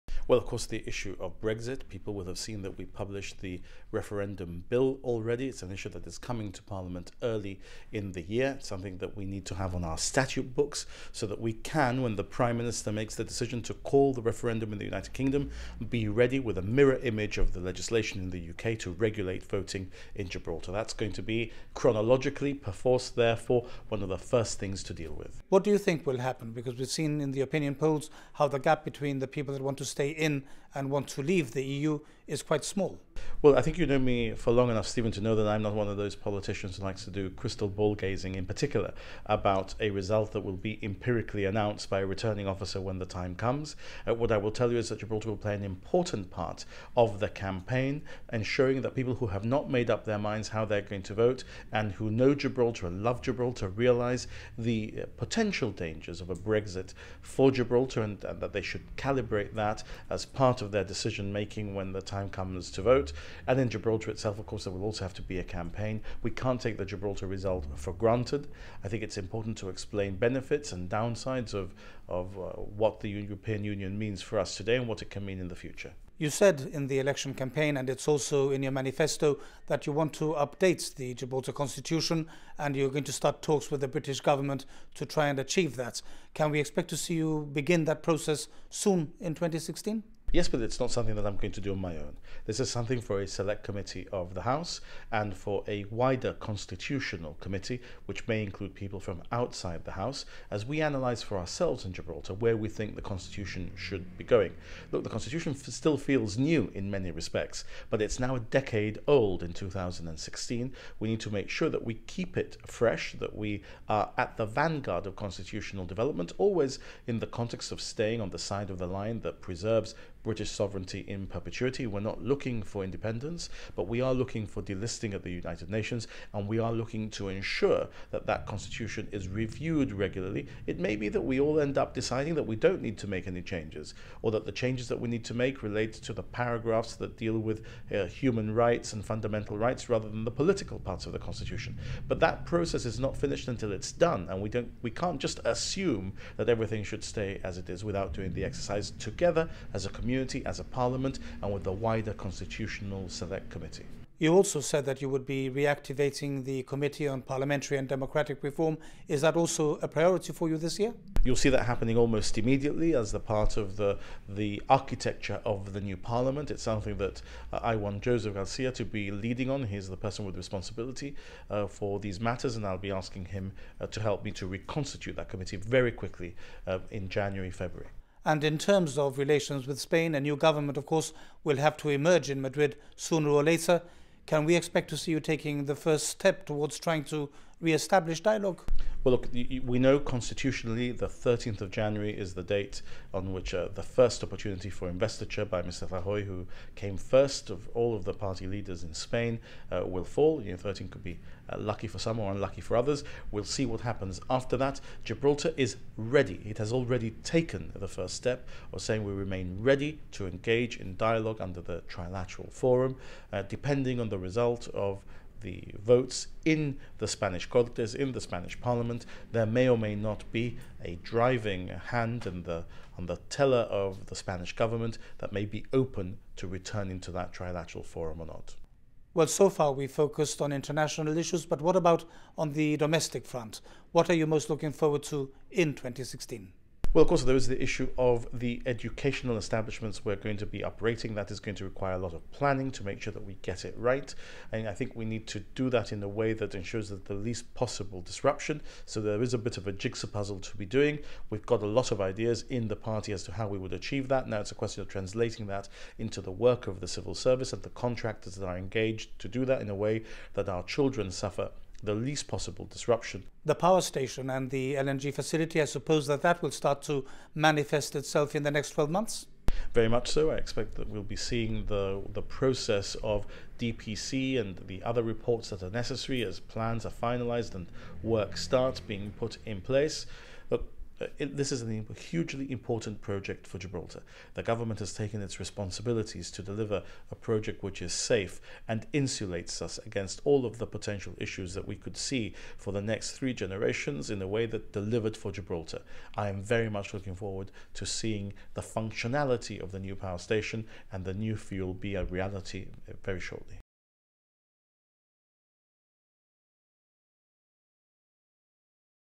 Fabian Picardo looks ahead to 2016 04.01.2016
Gibraltar remains ready to talk with Spain under the Trilateral Forum for dialogue. That's the message from Chief Minister Fabian Picardo to Madrid as it struggles to form a new government following last month's inconclusive general election. Speaking to GBC Mr Picardo noted that the investiture date for a new Spanish Prime Minister is  the 13th of January, and said that may be lucky for some and unlucky for others.

He revealed that the Select Committee on Parliamentary and Democratic reform will be reconstituted "almost immediately". The schools' upgrade will also be a priority, with a lot of planning required to minimise disruption.  The Chief Minister spoke to our news editor Stephen Neish, who began by asking him what was first in his in-tray for the New Year.